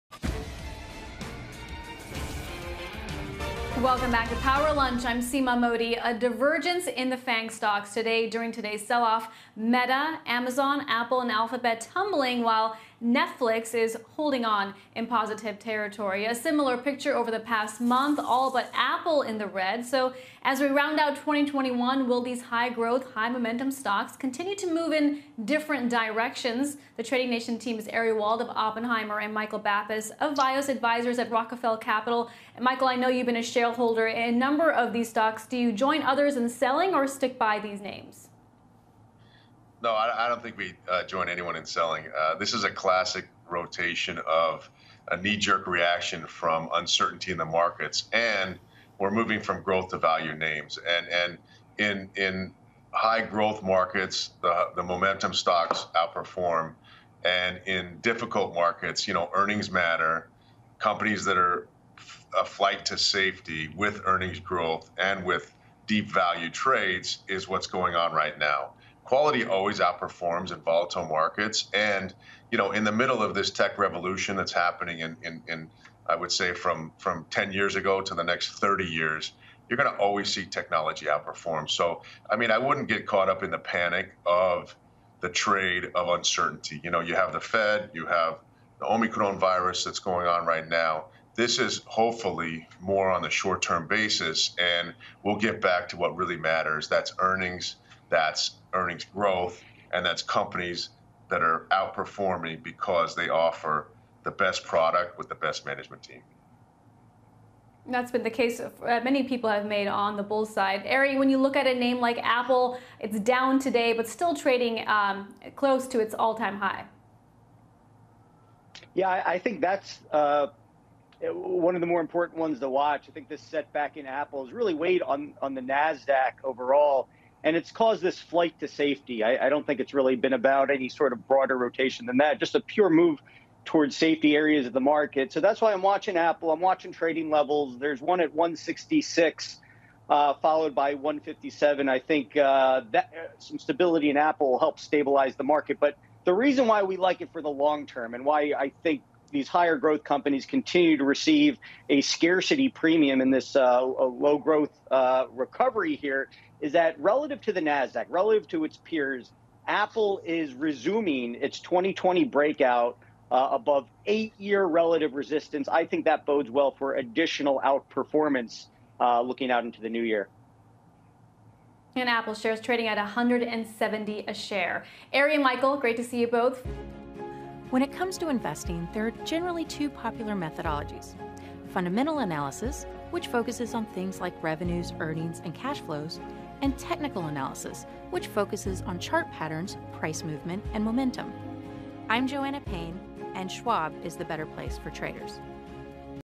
Tech stocks will always outperform: Investor
Michael Bapis of Vios Advisors and Ari Wald of Oppenheimer discuss FAANG stocks with CNBC's Seema Mody.